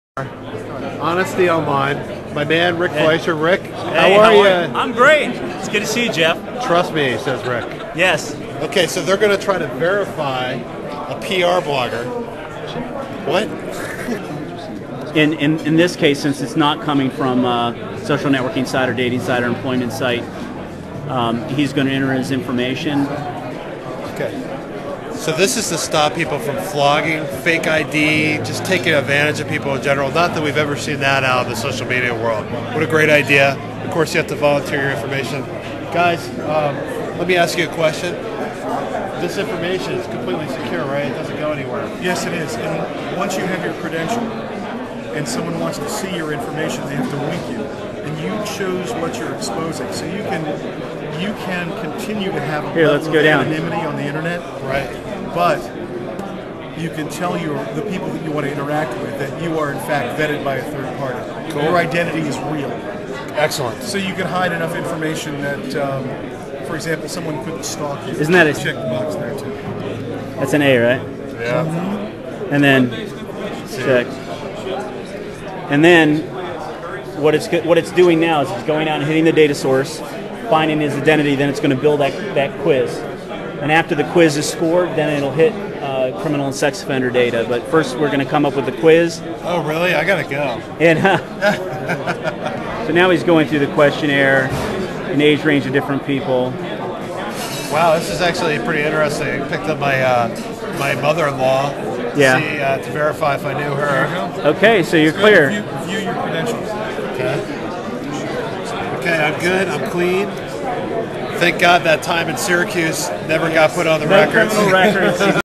honestyonline verifies Geoff Livingston's ID
Identity and transparency are increasingly important to building trust online.  Geoff Livingston demos honestyonline using his own ID.